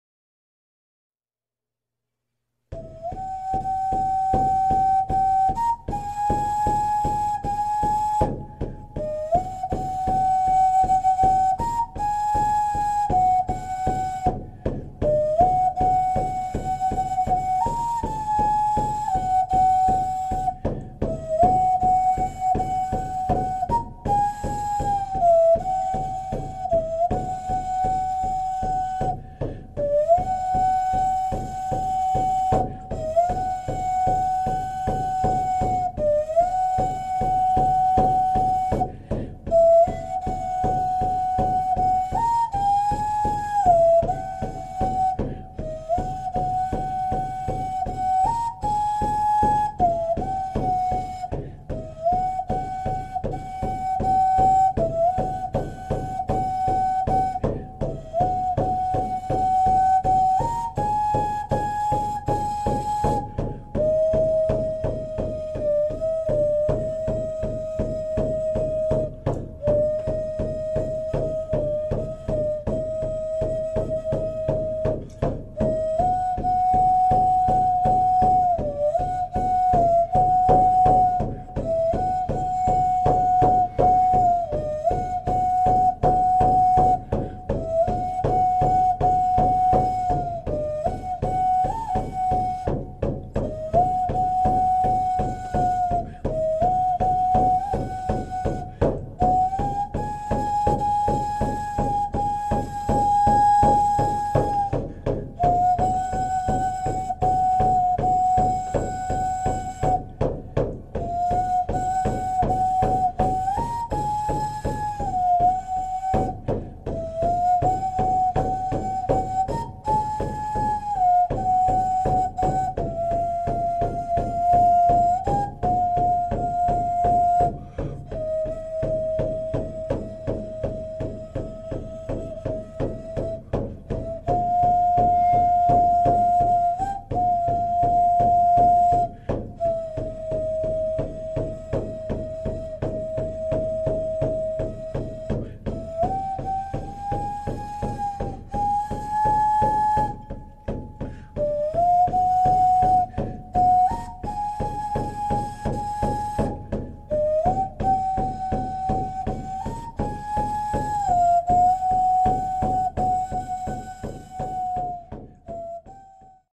Skull Whistle and Drumming a short meditation on change of season
Skull whistle played in unison with rhythmic drumming, photos taken in early Autumn near Schuylkill River, PA.
Skull whistle made from clay.